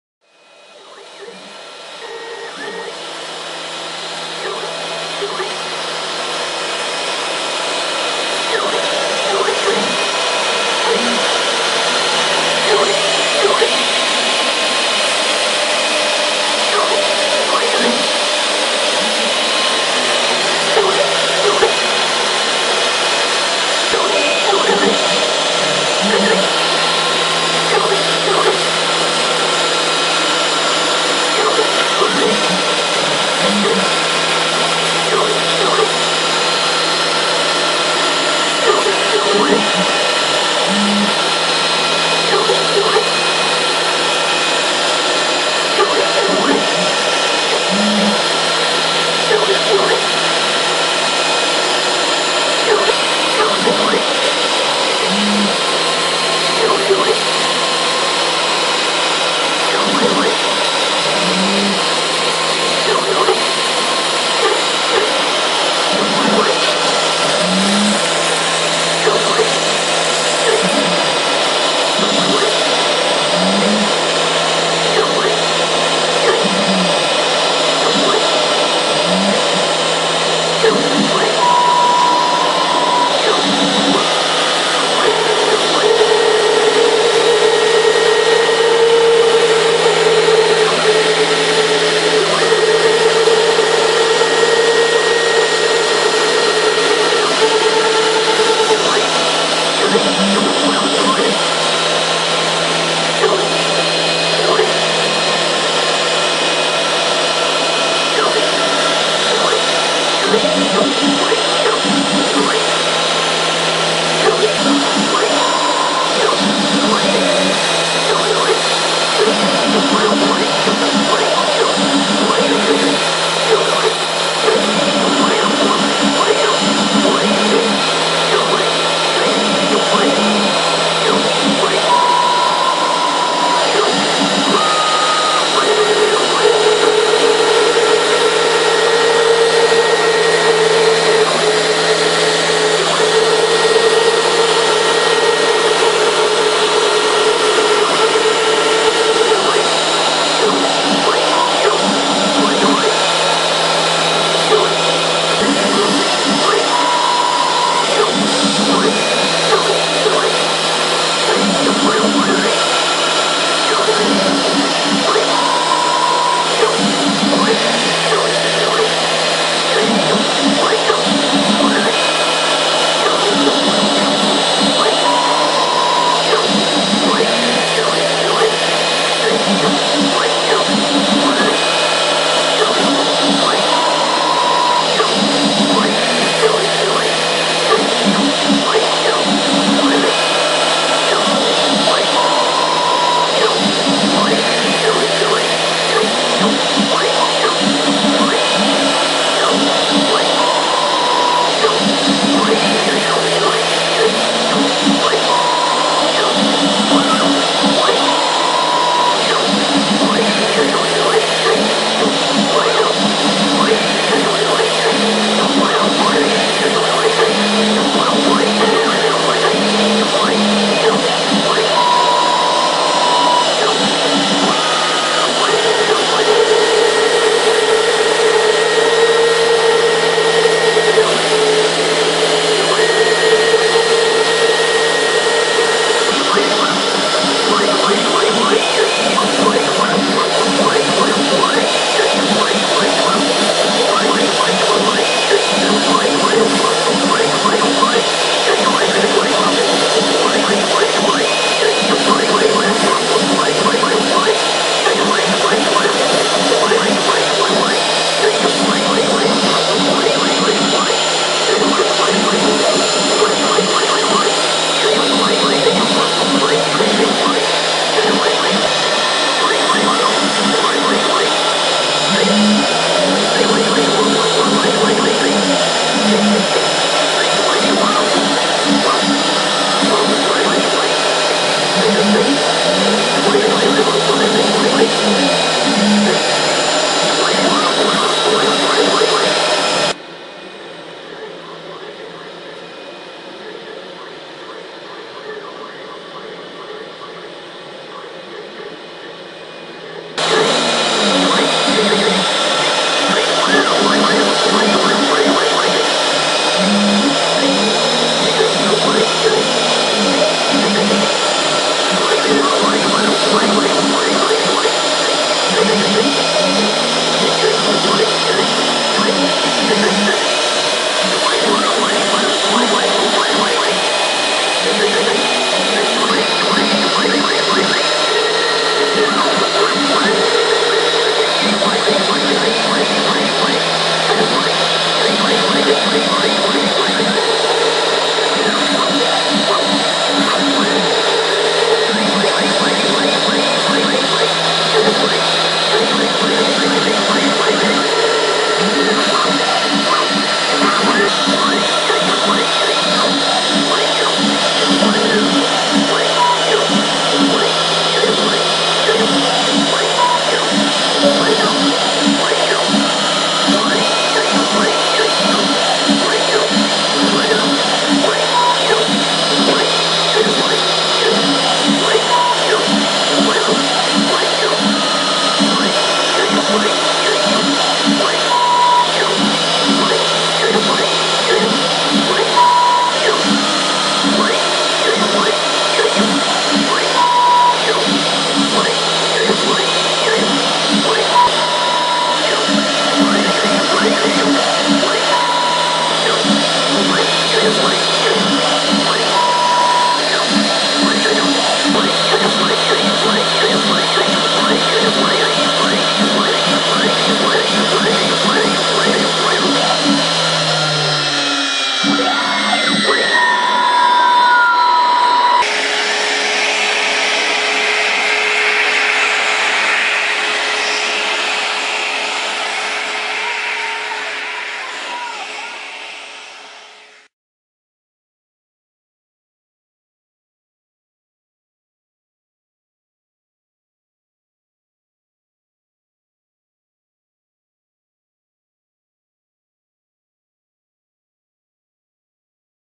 CNC-Holzfraese 3D Fraesen
Hier fräse ich ein kleines 3D Modell einer Pitts Challenger, max Geschwindigkeiten bis 4m/min weiter Infos